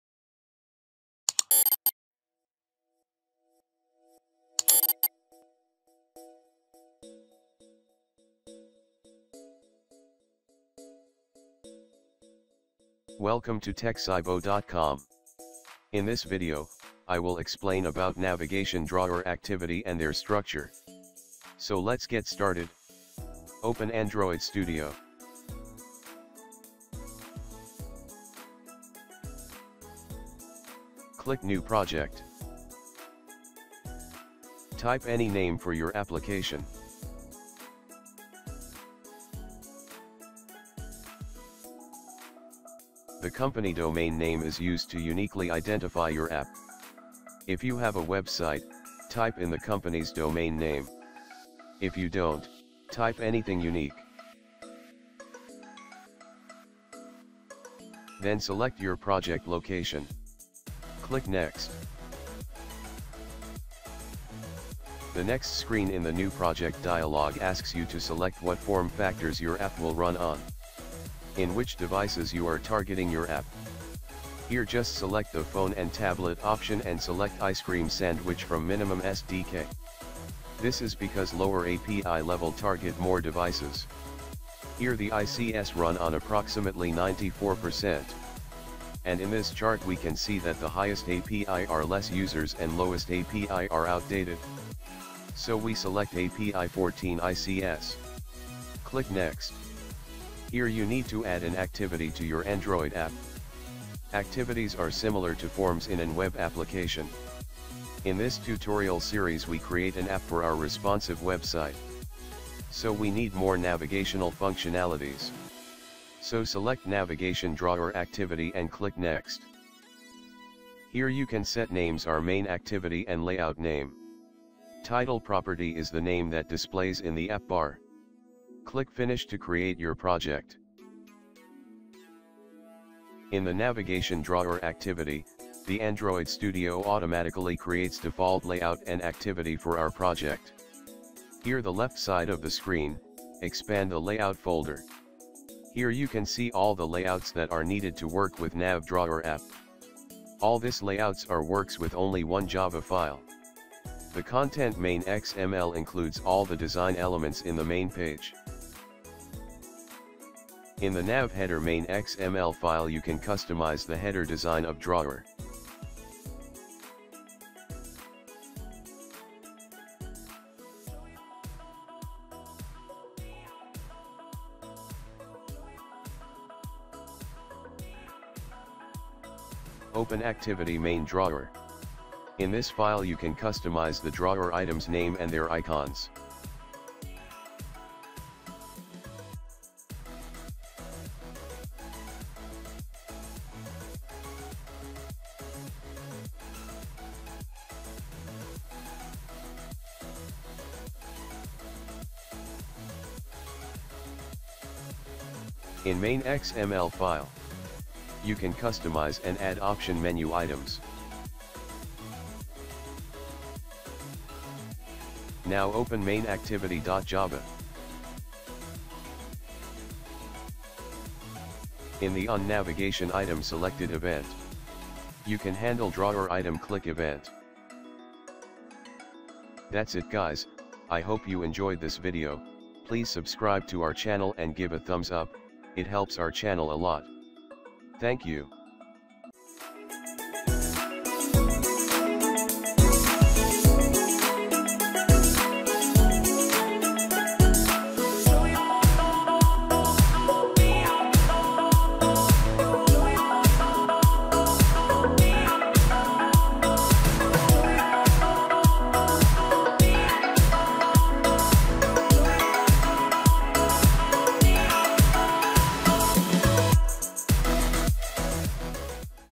Navigation Drawer Activity project | Android App from Responsive Website - 3
Creating Android App from Responsive Website with WebView

In this video i will explain Android Studio Template Navigation Drawer Activity

? Connect with NCS

? Follow Itro

? Follow Kontinuum